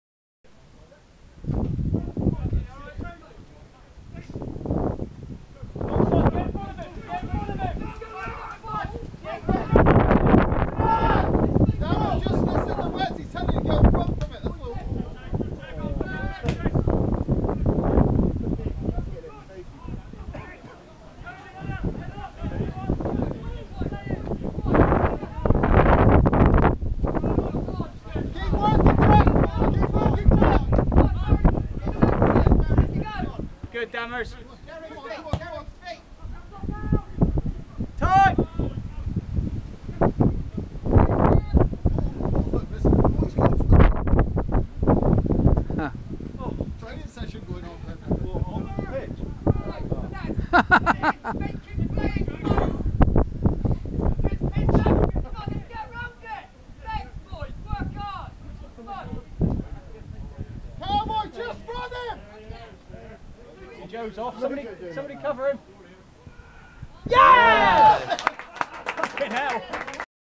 The KMD Cup 1st Round game, St Stephen 3 v 1 Veryan
match highlights Courtney Rowe puts defender Joe Stone through on goal and he runs on to fire a shot passed the Veryan keeper into the far corner, 3 - 0 on 52 minutes.